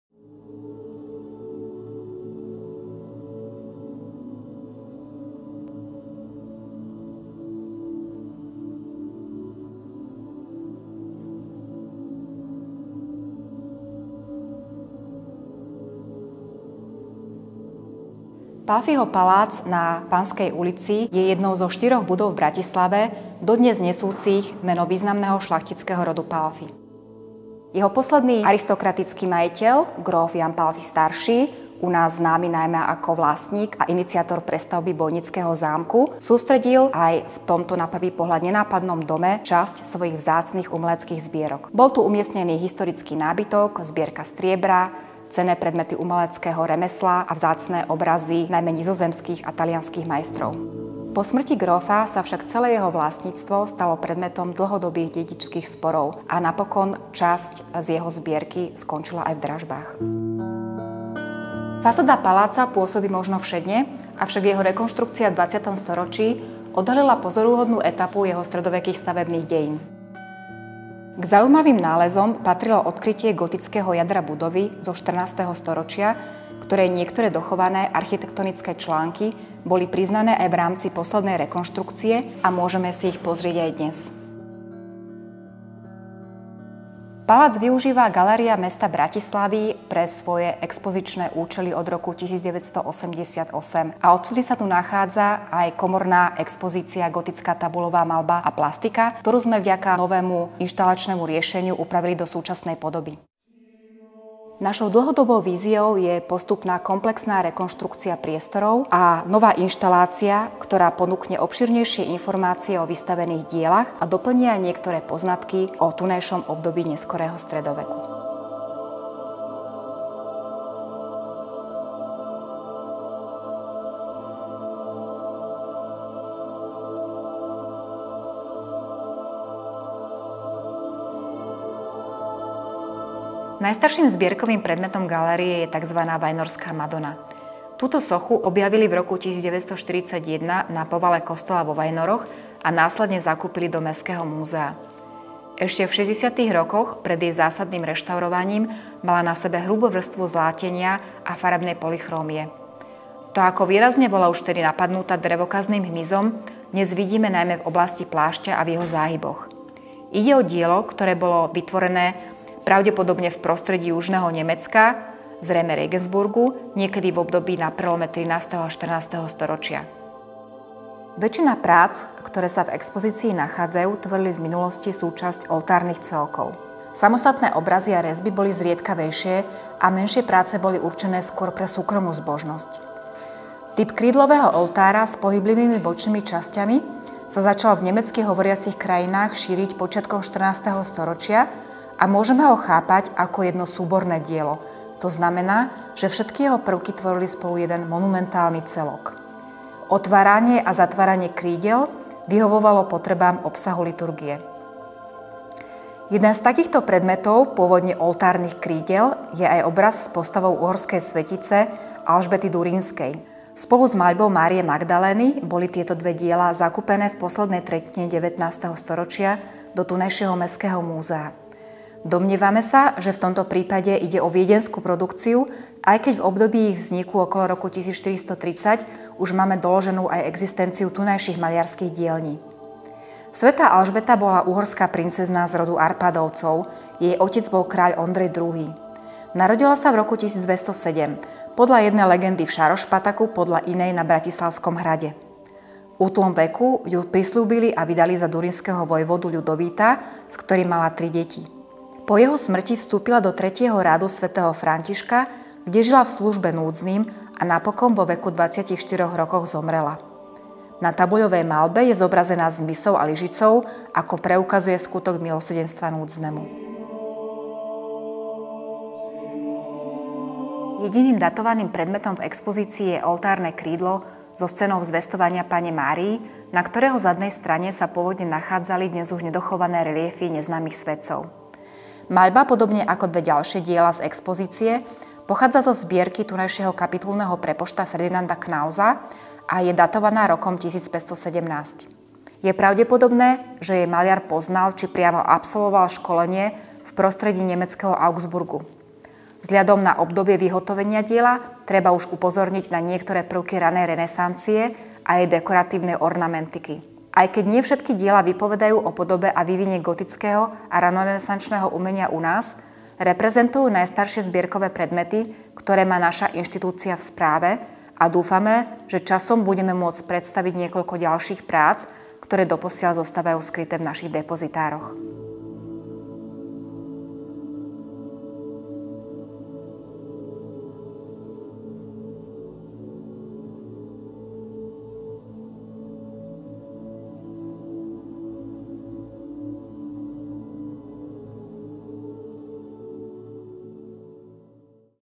Gotická tabuľová maľba a plastika | Stála expozícia
Pálffyho palác

Kurátorka: Mgr. Jana Luková
Pôvodná autorská koncepcia: Želmíra Grajciarová

Komorná zbierková expozícia Gotická tabuľová maľba a plastika v Pálffyho paláci bola otvorená v roku 1988 po predchádzajúcej celkovej rekonštrukcii budovy a jej sprístupnení verejnosti pod správou Galérie mesta Bratislavy. Všetky diela získala galéria zo zbierok Múzea mesta Bratislavy a väčšinu predstavila už v roku 1973 v osobitnej expozícii v kostole klarisiek, ktorý slúžil a dodnes slúži aj ako polyfunkčný kultúrny priestor.

Vzhľadom na umiestnenie v mestských zbierkach bola mnohým predmetom pripisovaná tunajšia proveniencia. Dôkladnejší výskum však časom ukázal, že sú to pôvodom rôznorodé diela sakrálneho umenia pochádzajúce z významných lokálnych zbierok. Ide zväčša o fragmenty nedochovaných oltárnych architektúr, ktoré si už dnes v pôvodnej podobe nevieme dobre predstaviť a ktoré časovo radíme do obdobia od prelomu 13. a 14. storočia až po prvú tretinu 16. storočia. Aj keď nie všetky vypovedajú o podobe gotického a ranorenesančného umenia u nás, reprezentujú najstaršie zbierkové predmety, ktoré má naša inštitúcia v správe. Za všetky vyzdvihnime sochu Vajnorskej madony s dieťaťom – rezbu z dubového dreva, ktorá bola vytvorená pravdepodobne v regensburskej dielni (Regensburg, východné Bavorsko) možno už na konci 13. storočia. Meno dostala podľa miesta nálezu pred zakúpením do múzea.

Expozícia, ktorú pripravila dlhoročná kurátorka zbierok PhDr. Želmíra Grajciarová, ostala počas svojej  vyše tridsaťročnej existencie v podstate nezmenená, ale v roku 2020 sme vďaka finančnej podpore Bratislavského samosprávneho kraja mohli začať s realizáciou súčasného koncepčného a vizuálneho riešenia novým rozmiestnením exponátov a výmenou niektorých inštalačných prvkov. Našou dlhodobou víziou je postupná komplexná rekonštrukcia priestorov a citlivá moderná inštalácia, ktorá ponúkne návštevníkovi obšírnejšie informácie o vystavených dielach a doplní niektoré poznatky o tunajšom období neskorého stredoveku.

➜ Mirbachov palác | Františkánske námestie 11
➜ Pálffyho palác | Panská 19

UT / 11:00 – 18:00
ST / 13:00 – 20:00
ŠT / PI / SO / NE / 11:00 – 18:00